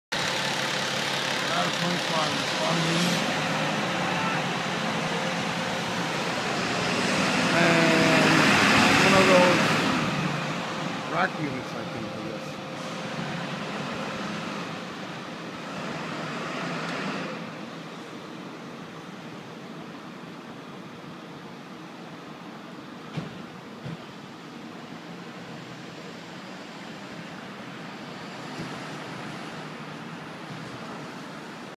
FDNY LADDER 25 AND FDNY SUPPORT UNIT GO OUT TO RESPOND TO A CALL ON UPPER WEST SIDE OF MANHATTAN.
HERE YOU WILL FDNY LADDER 25 AND FDNY SUPPORT UNIT GOING OUT TO RESPOND TO A CALL ON THE UPPER WEST SIDE OF MANHATTAN IN NEW YORK CITY.